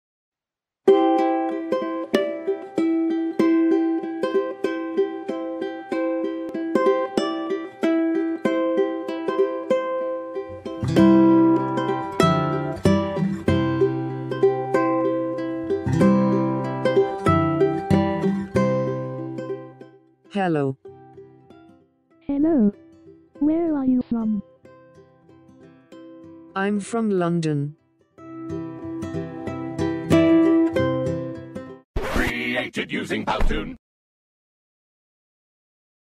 Where are you from?
Comment demander d’où quelqu'un viens en Anglais